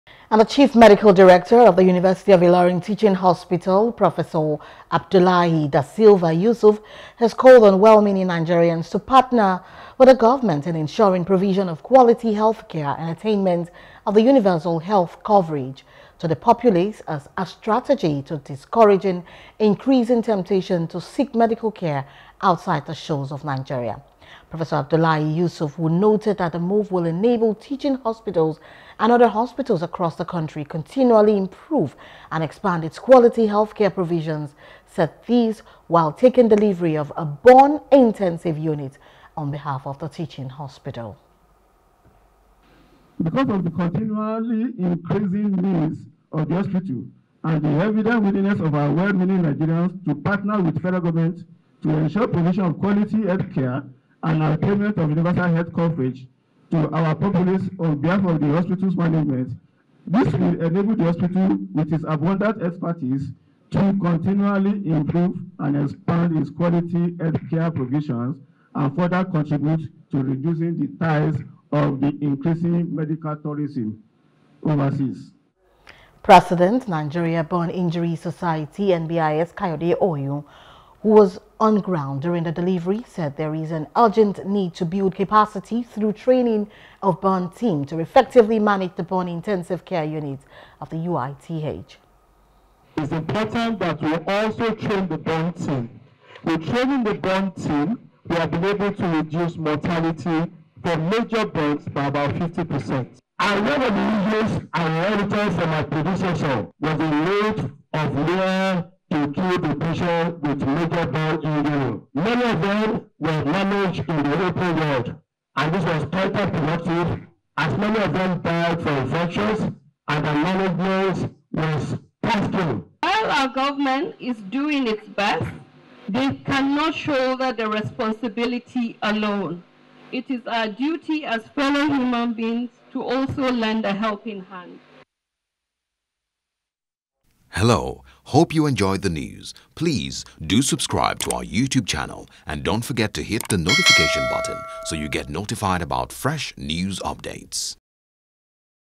Medical Tourism: Director Calls For Partnership
The Chief Medical Director of the University of Ilorin Teaching Hospital (UITH), Professor Abdullahi Dasilva Yussuf has called on well-meaning Nigerians to partner government in ensuring the provision of quality healthcare and attainment of Universal Health Coverage (UCH) to the populace as a strategy to discouraging increasing temptation to seek medical care outside the shores of Nigeria. Professor Abdullahi Yusuf who noted that the move will enable teaching hospitals and other hospitals across the country to continually improve and expand its quality healthcare provisions said this while taking delivery of a burn intensive unit on behalf of the teaching hospital.